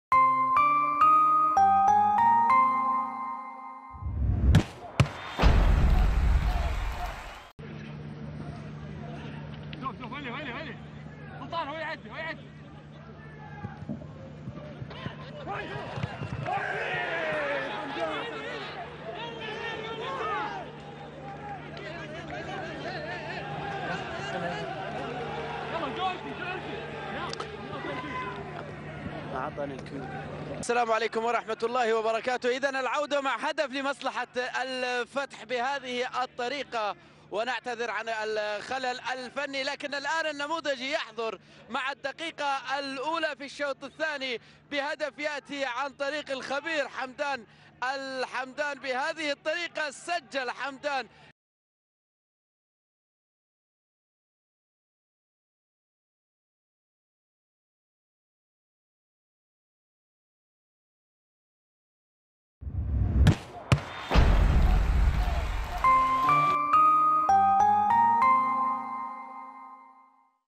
هدف الفتح الأول ضد الباطن (حمدان الحمدان) في الجولة 10 من دوري جميل
تابع مباريات دوري_جميل مباشرة و حصريا على موقع وتطبيق دوري_بلس